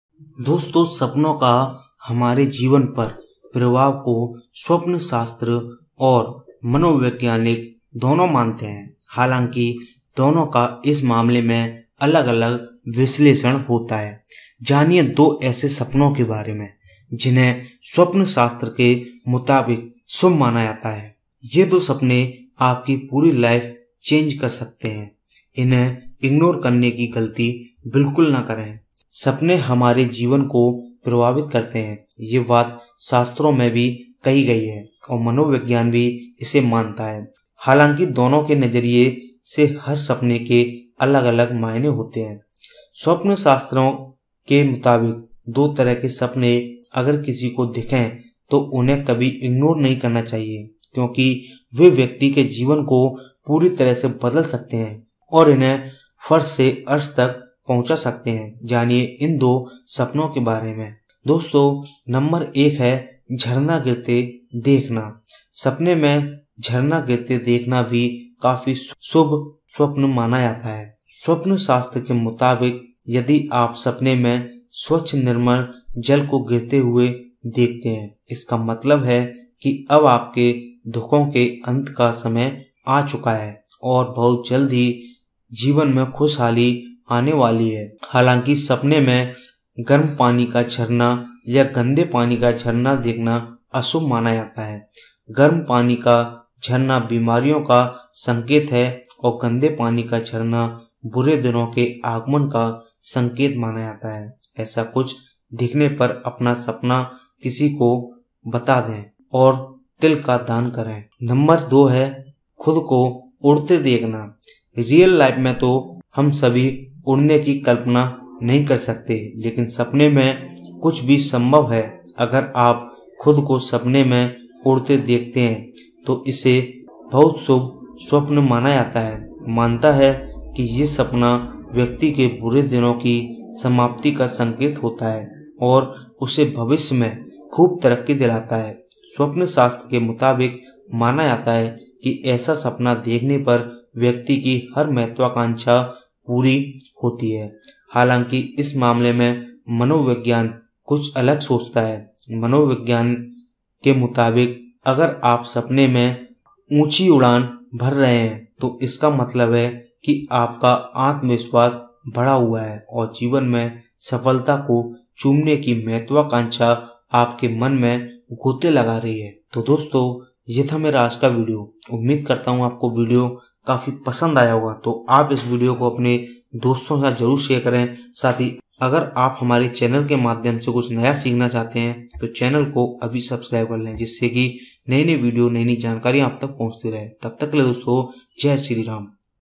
सपने में ये 2 चीज दिखे तो समज जाना आपकी लॉटरी लगने वाली है Vastu shastra
सपने में ये 2 चीज दिखे तो समज जाना आपकी लॉटरी लगने वाली है . वास्तु शास्त्र Vastu Shastra,  Jyotish Shastra| Chanakya Neeti Motivational | Chanakya Niti full in hindi | Motivational Video